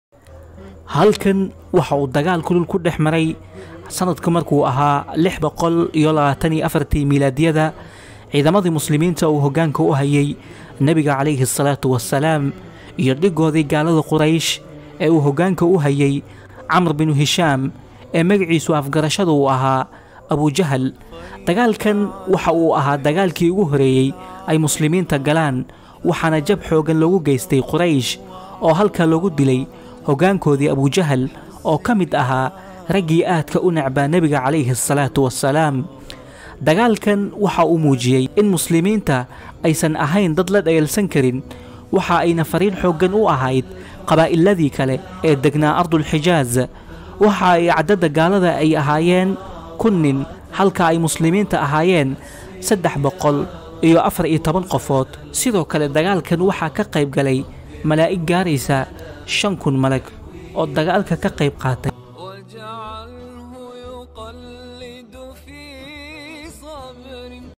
GOOBTII UU KADHACAY DAGAALKII BADAR
HALKA WAA MEESHII UU KADHACAY DAGAALKII BADAR